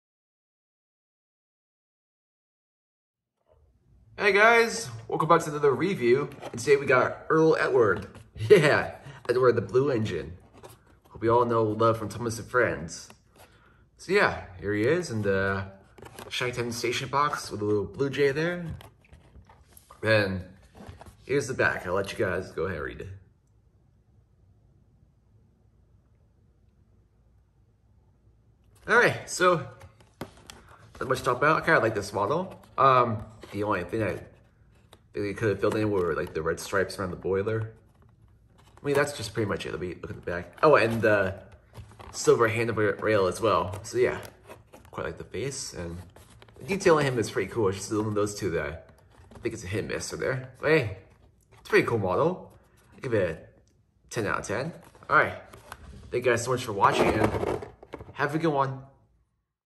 Thomas and Friends: ERTL Edward Review
A review of the ERTL Edward from ERTL.

By the time this review goes up, the item was already sold to a friend.